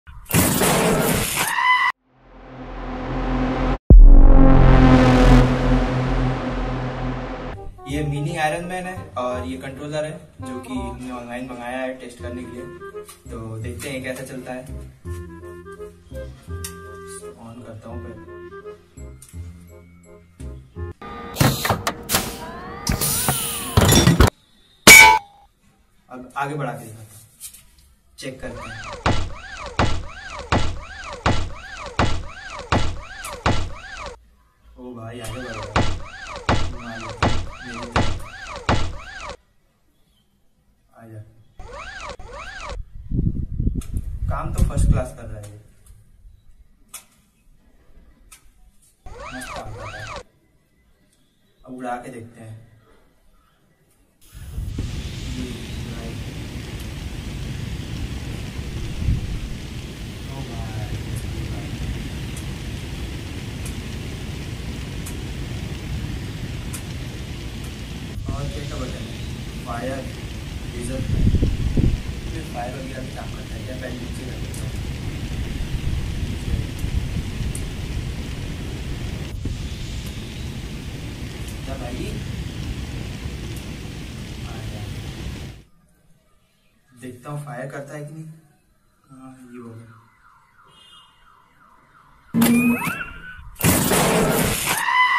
Iron Man Suit VFX!
Iron Man Suit Remote Control Test VFX
Hi Folks,
This Is Animated Video In Multiple Software You Can't Find it Online 😂.
Hope Accha Laga Hoga 😗